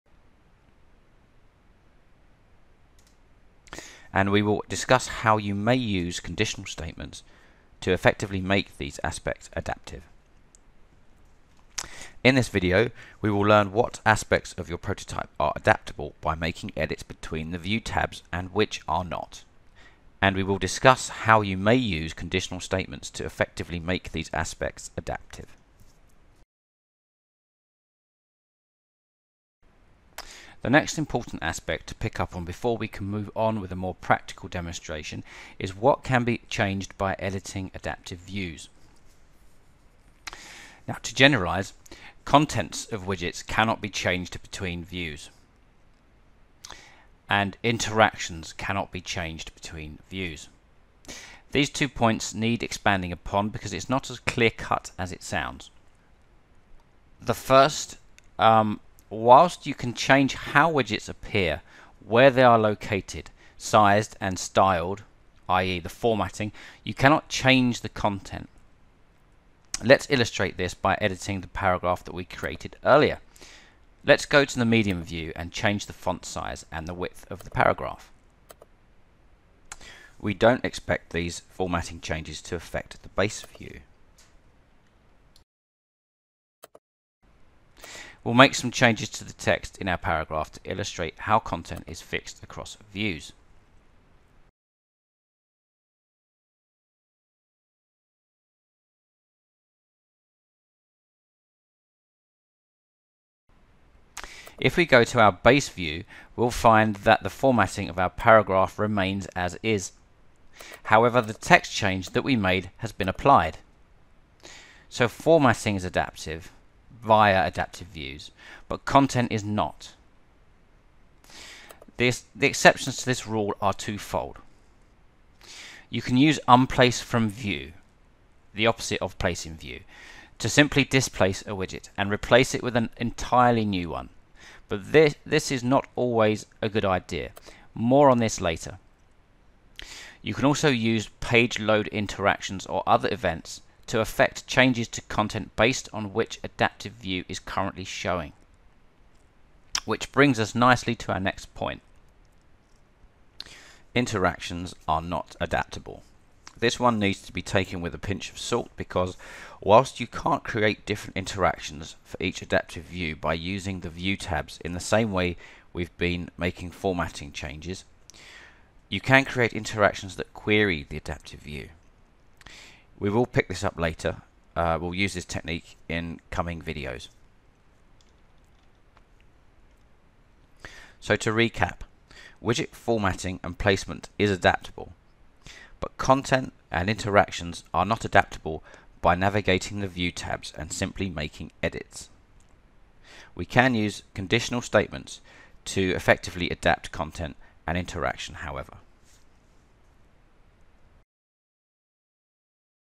6d. Everything You Need to Know about Adaptive Views
Part 6: Designing a Responsive Site Framework with Adaptive Views

Get professional axure training, prototype your idea, and have your prototype coded